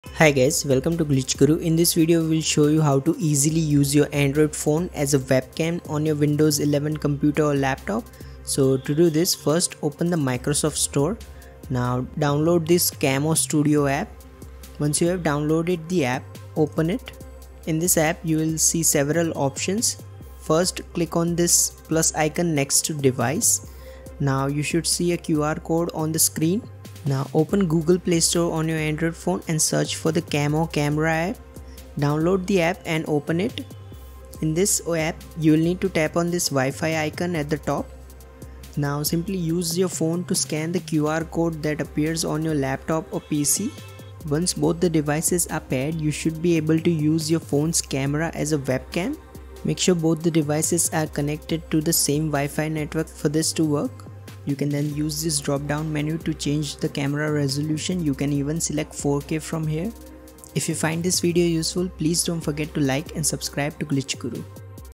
How to Use Android Phone as Webcam on Windows 11 (4K Quality)
Want to use your Android phone as a webcam on Windows 11—completely free? In this video, we’ll walk you through exactly how to use your phone as a webcam for your PC wirelessly.

My Favorite Laptop (Amazon)
Laptop Stand (Amazon)

We’ll explore the best methods to use an Android phone as a webcam on Windows 11, including both wired and wireless options.

You’ll learn how to connect your phone camera to your laptop, how to install the necessary apps, and how to configure everything so your PC recognizes your phone as a webcam.